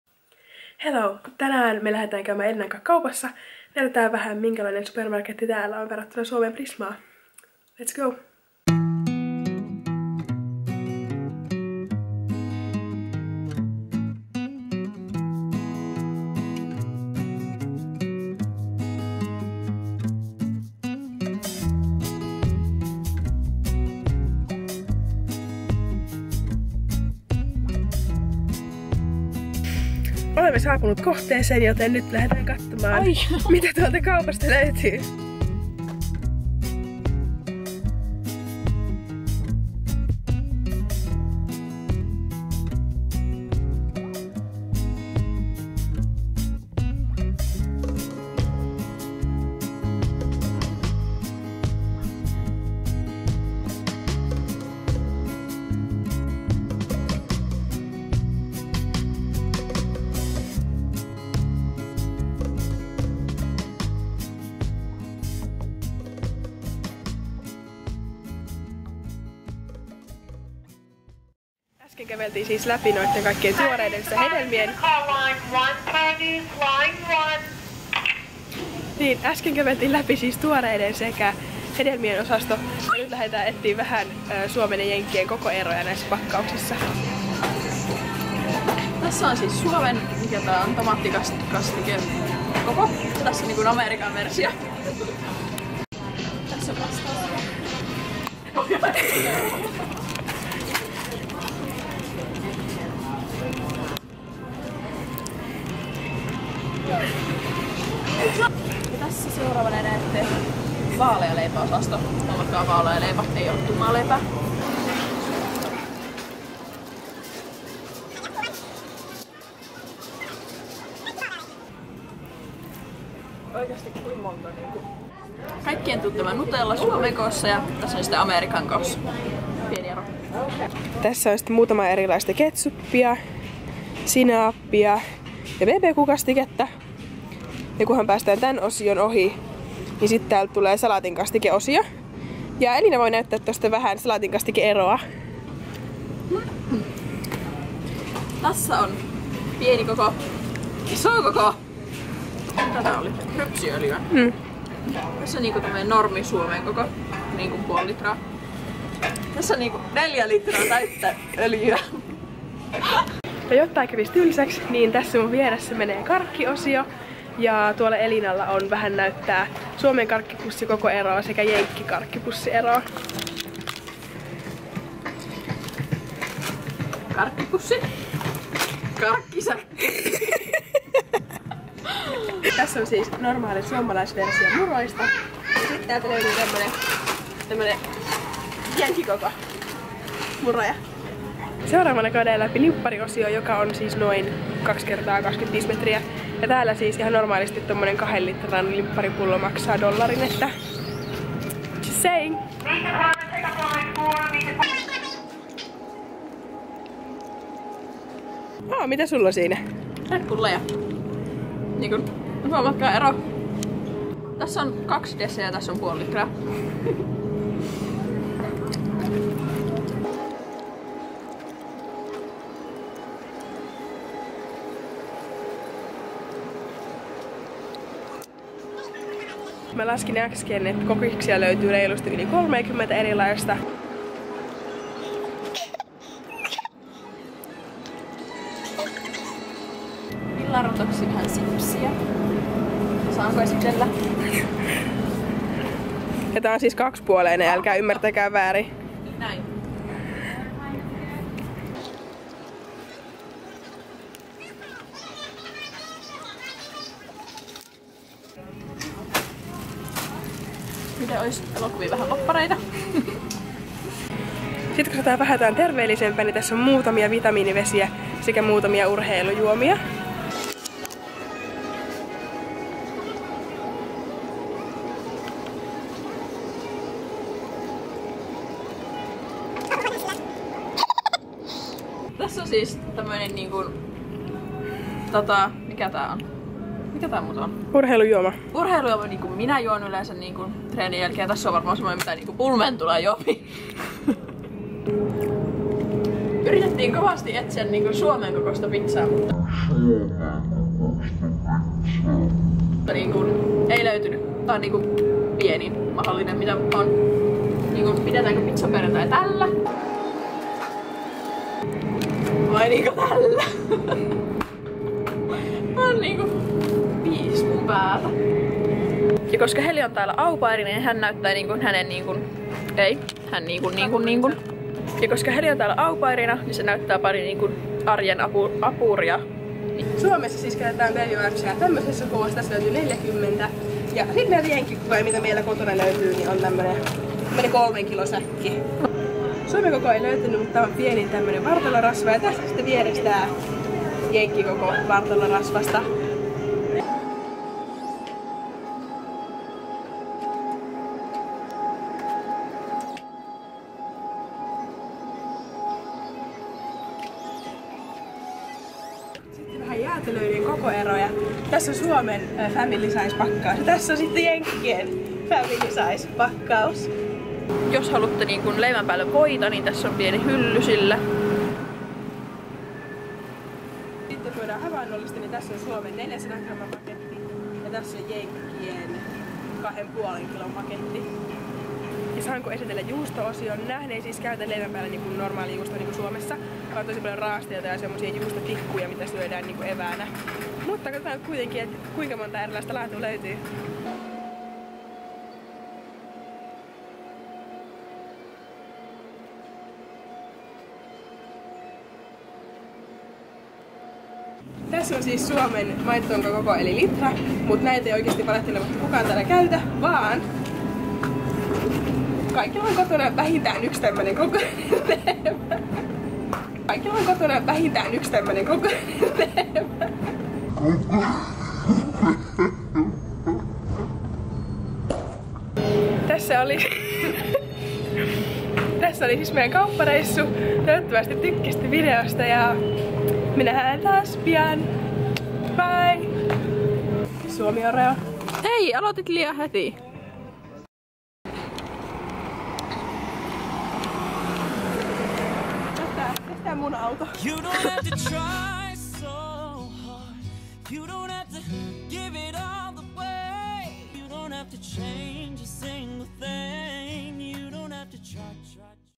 🇫🇮🇺🇸Kaupassa: Usa vs Suomi
There is subtitles in English, but don't watch em too closely.
Writing in English is not my strongest area..

Elina tulee näkymään muutamalla mun seuraavalla videolla, koska oli täällä mun luona muutaman viikon käymässä.
Plus huomatkaa, että ei olla oltu piiiiiiiitkään aikaan kahta viikkoa saman katon alla, eikä edes nähty useampaan kuukauteen.

Anyways, nauttikaa videosta
ja heittäkää kommenttiä tai näyttäkää mulle peukuilla mitä mieltä olitte videosta!

Love you.

plus Snapchat: helistiina
Elinan SnapChat: elinazx